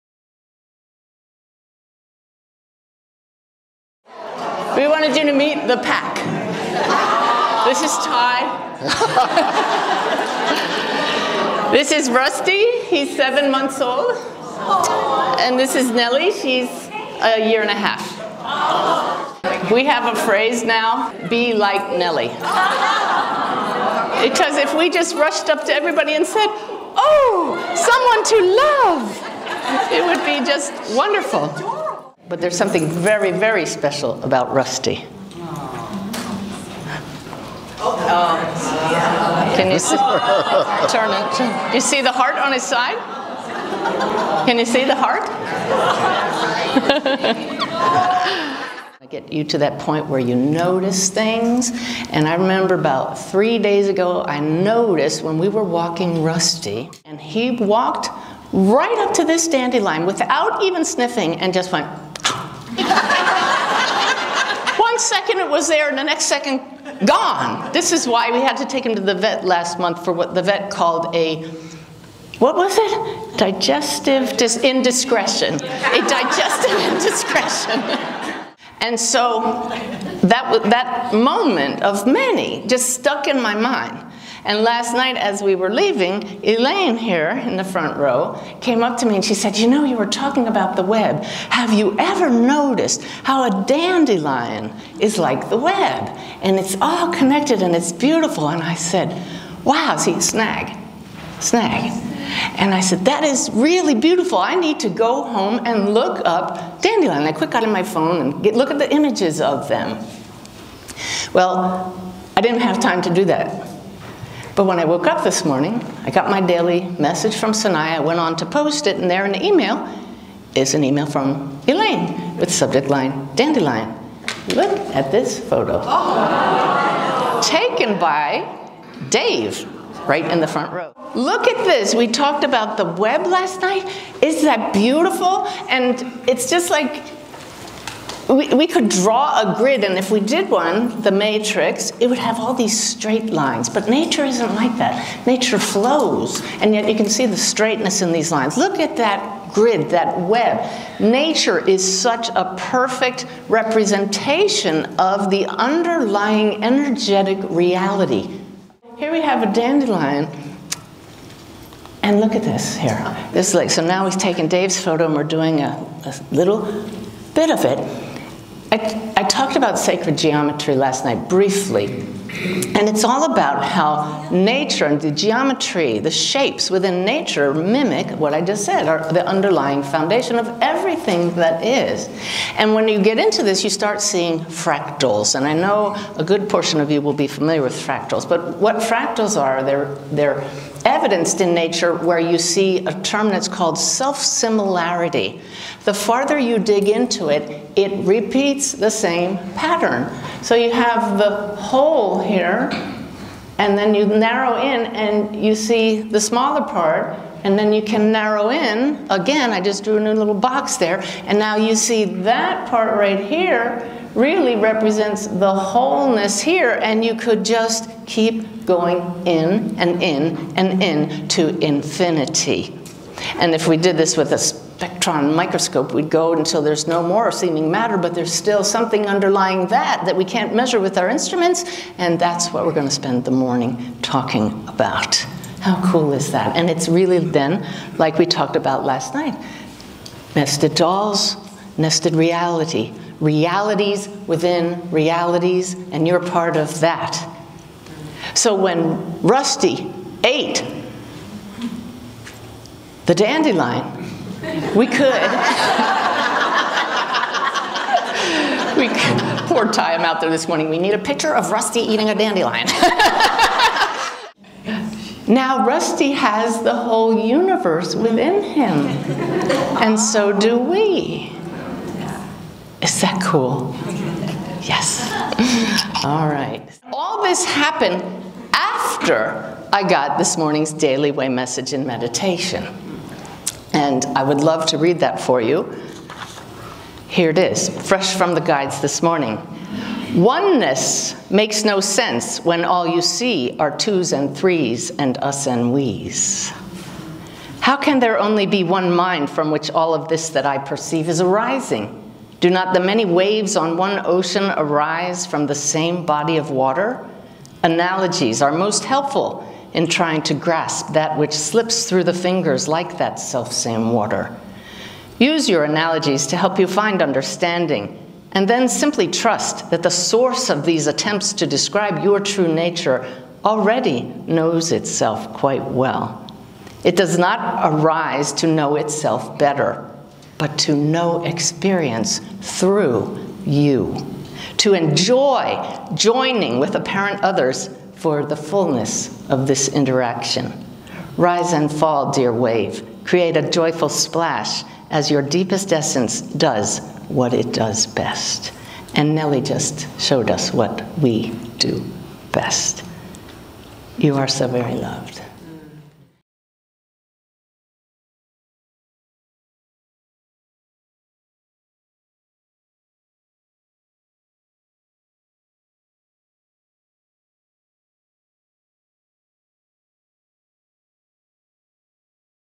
Seeing the Oneness
Messenger of Hope Suzanne Giesemann shares this clip about oneness from her W-Holy You retreat.  Hear how the Giesemann's two dachshunds spread love and their antics lead to an excellent explanation of our True Nature as expressions of one Mind ... of Consciousness.